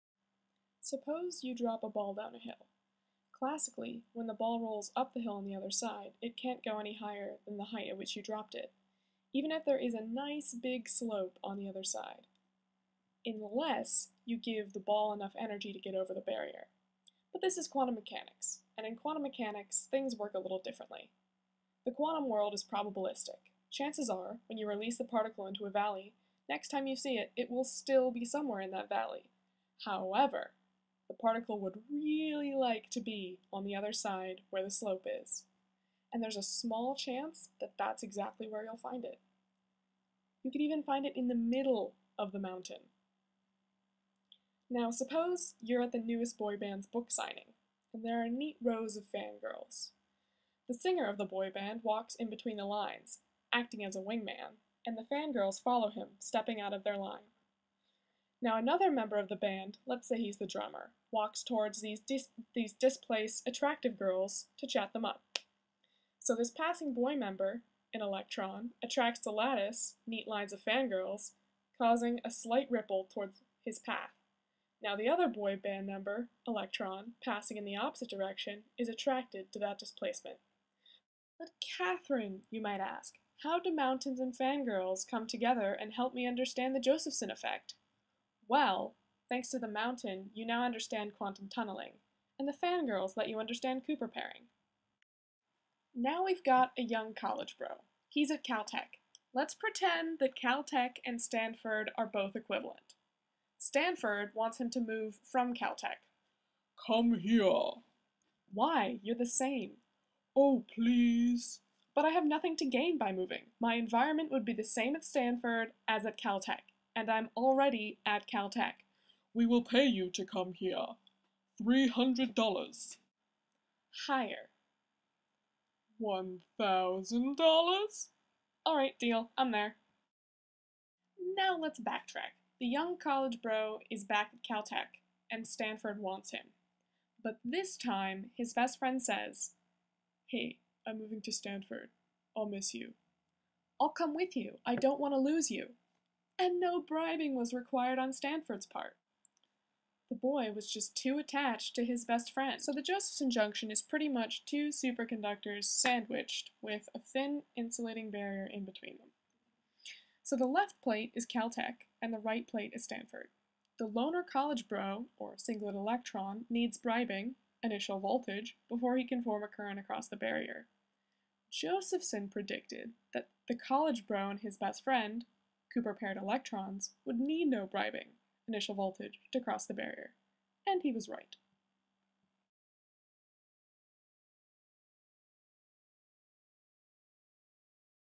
A Short Explanation of the Josephson Effect
Quantum tunnelling (or tunneling if you're from the UK), Cooper pairing, and Josephson junctions, oh my!

All credit for the mountain analogy (including those drawings) goes to minutephysics from his video on Quantum Tunneling.